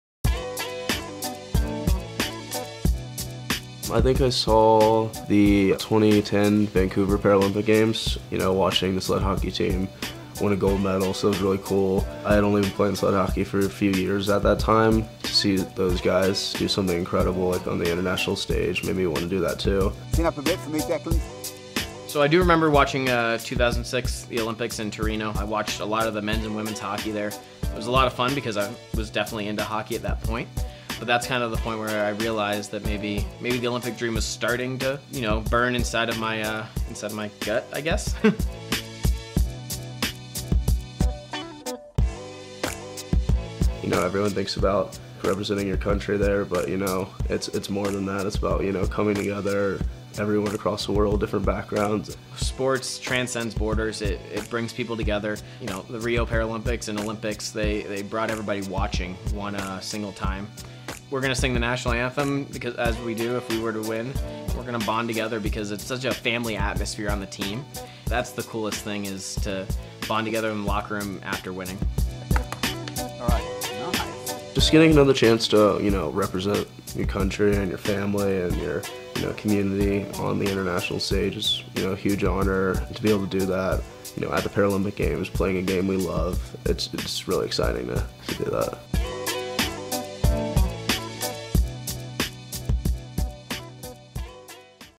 Sled Hockey's Declan Farmer & Josh Pauls Talk Unity At The Paralympic Games | Meet Team USA | TIME
Team USA sled hockey players Declan Farmer and Josh Pauls take you inside their photoshoot before the 2018 PyeongChange Winter Olympics.